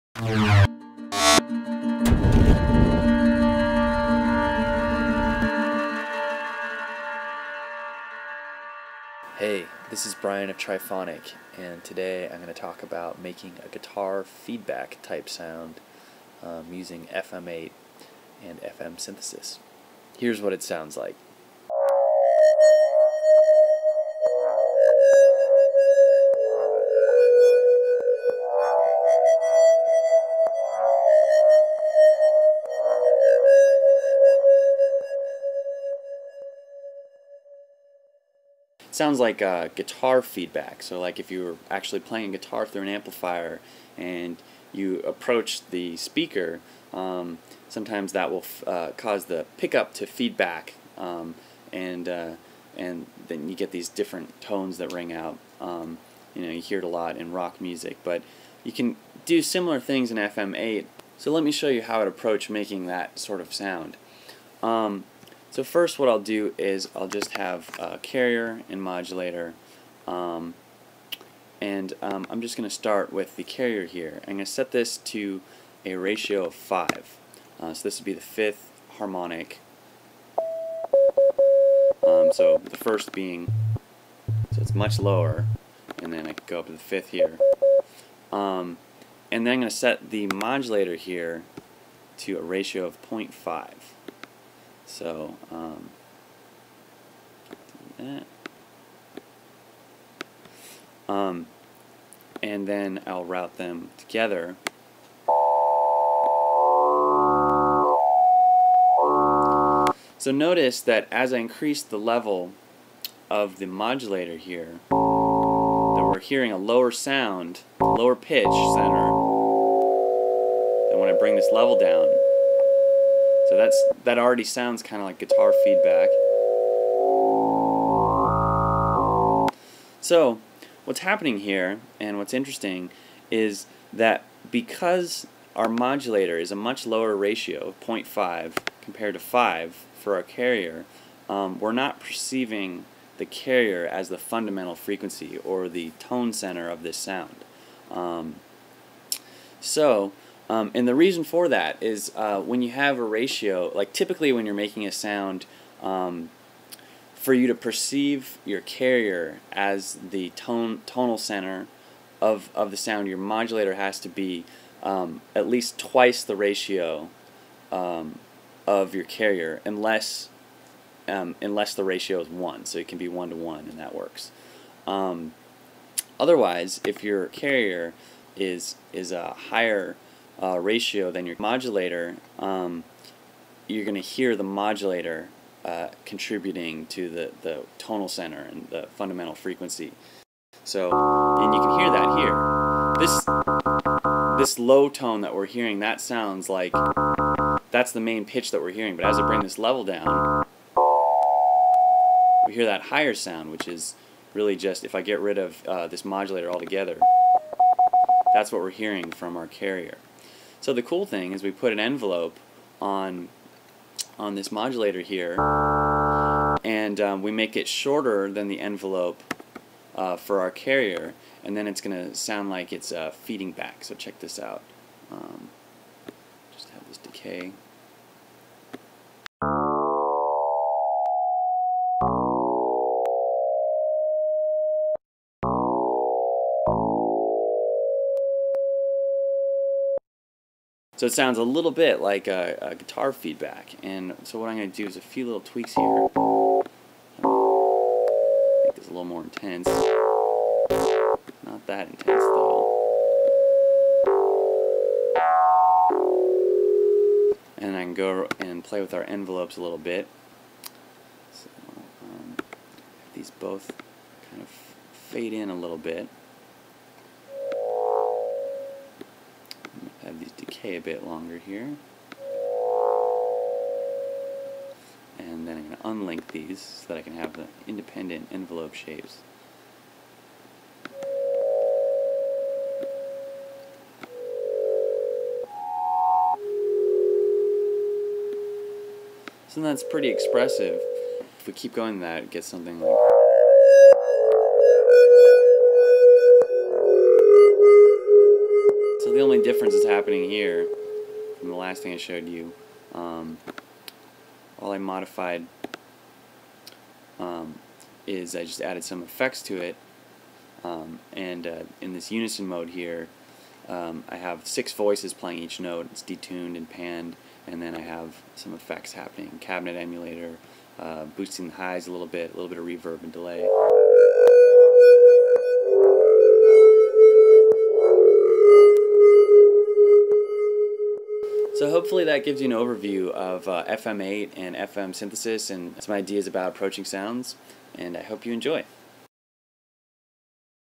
Trifonic: FM Synthesis Basics Pt 3
Brian is back one more gin to break down how FM synthesis works. In this episode he demonstrates how to create a guitar-like feedback sound using FM8 and the principles of FM synthesis.